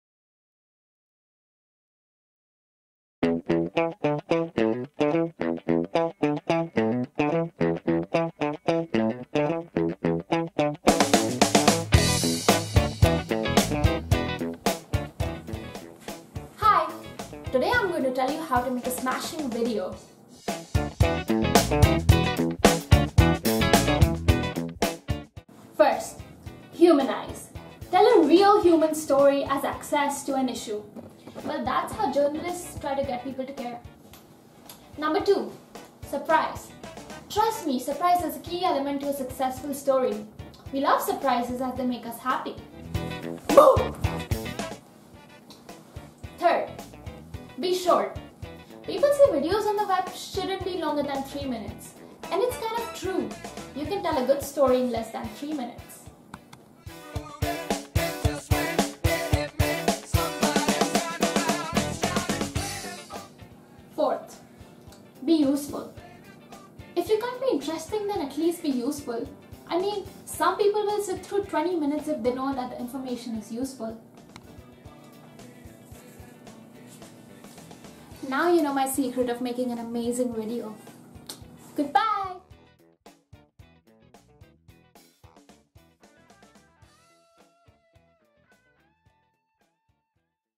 Vlog 00-01
My first Vlog about "How to make an amazing video"